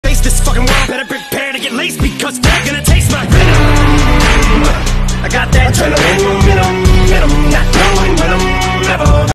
He saved a girl's life!
The man was giving an interview when he saw that a hockey puck was flying straight at the girl. Caught the puck like Bruce Lee!